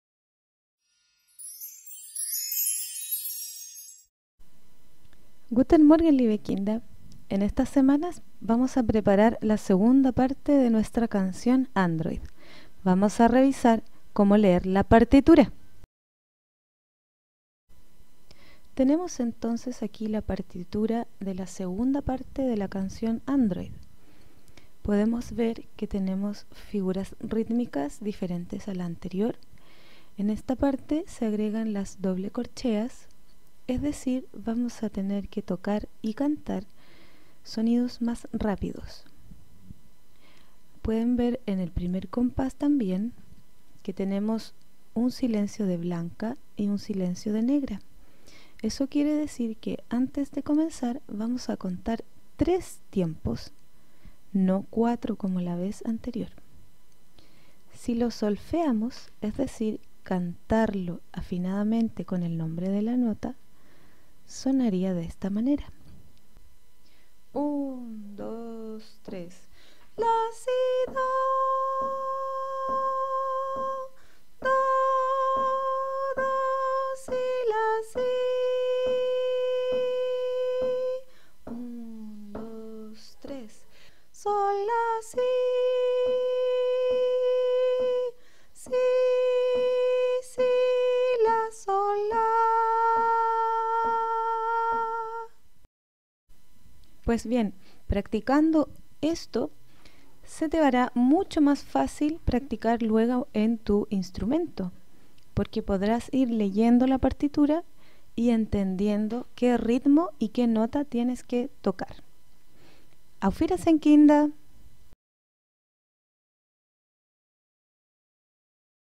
Leo la partitura de Android segunda parte
Actividad preparada para estudiantes de 4º básico del Colegio Alemán de Los Ángeles.